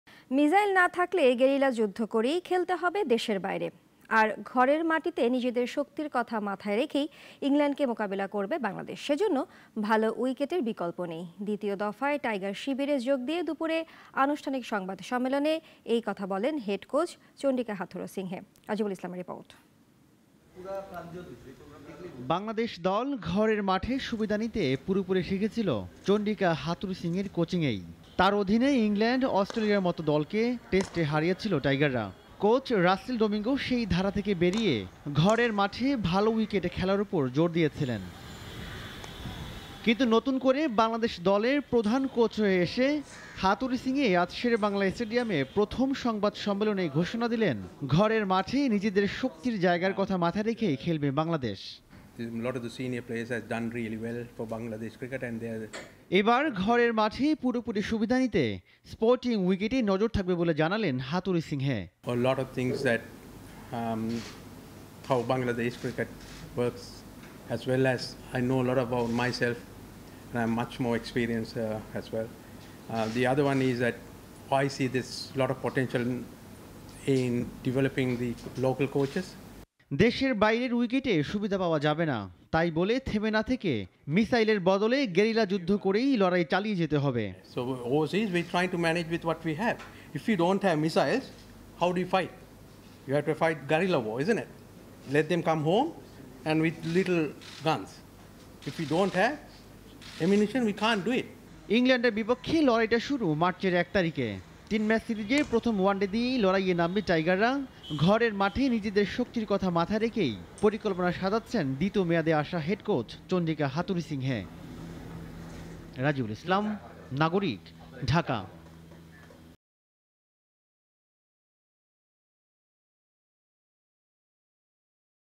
মি*সাইল না থাকলে গেরিলা যু*দ্ধ করতে হবে: হাথুরুসিংহে | Cricket | BD Coach | Nagorik TV
মি*সাইল না থাকলে গেরিলা যু*দ্ধ করতে হবে: হাথুরুসিংহে